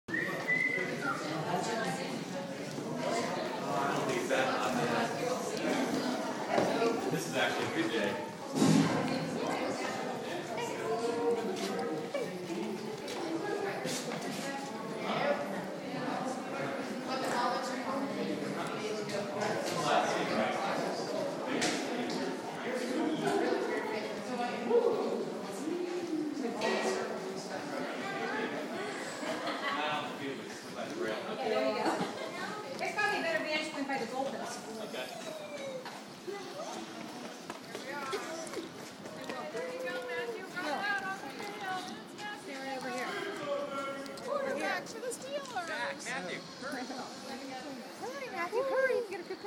Walk from Steelers locker room to the field
This is the walk that the steeler players make when they leave the locker room to go out on the field. @sn0w13allz